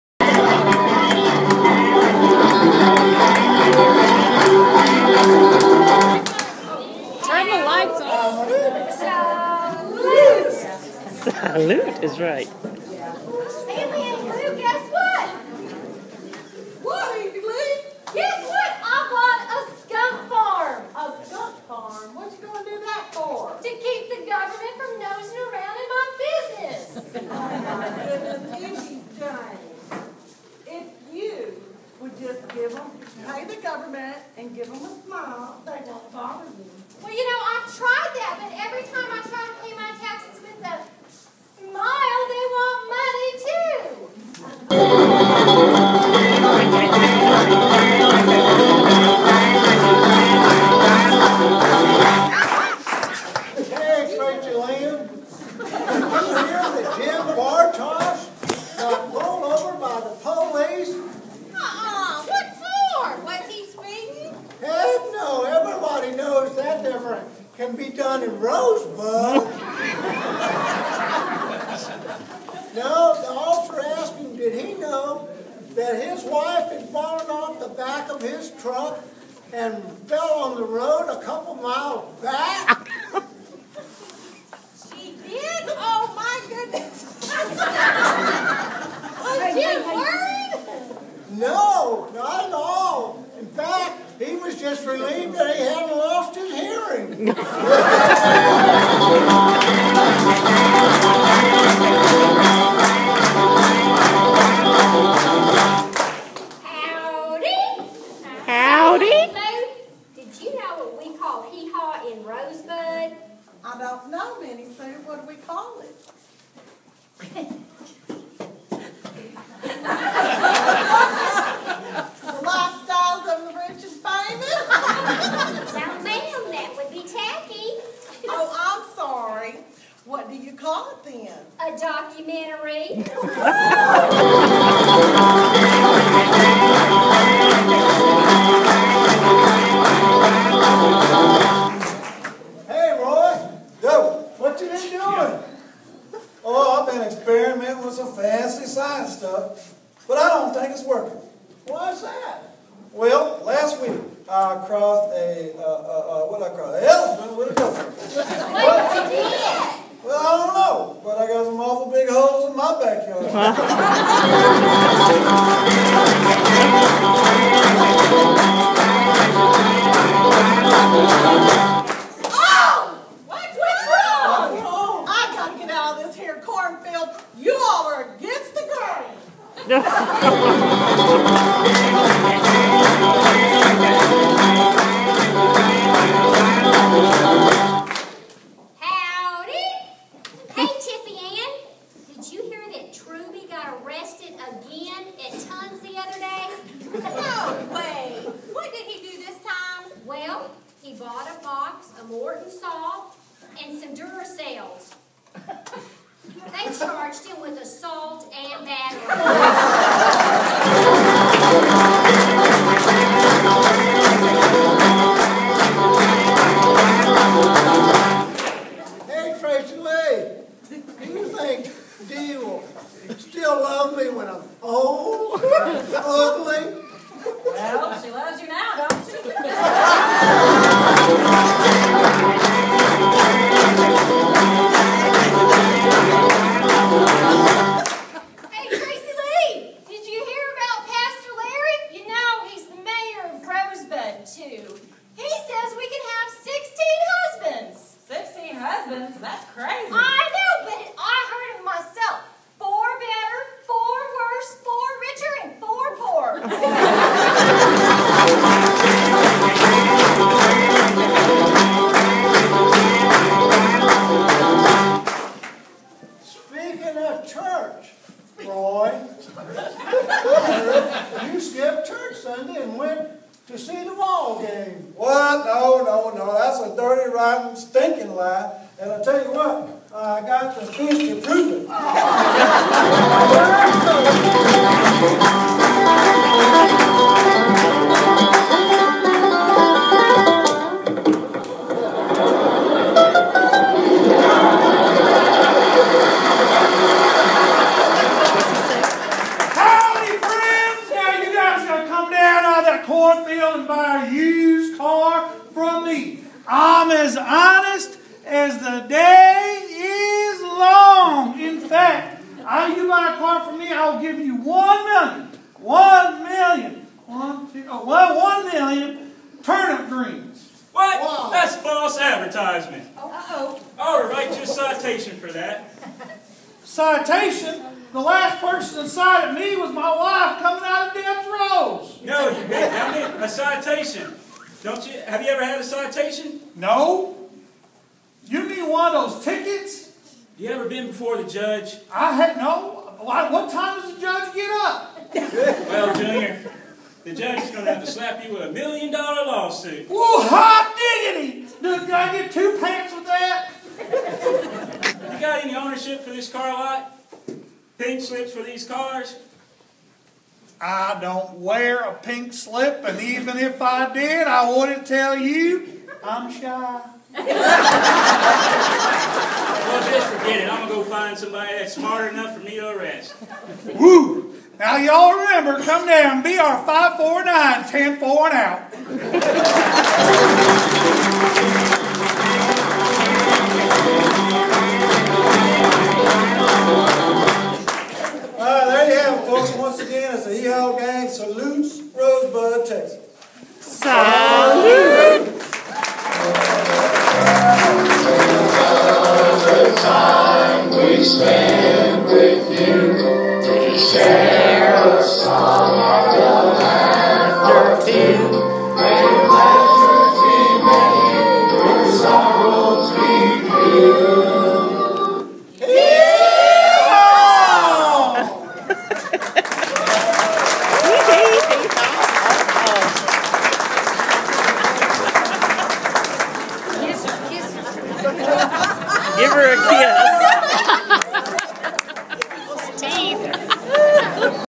HeeHawSkit
Rosebud Chamber of Commerce and Agriculture - April 2013 Annual Banquet.  Hee Haw Skit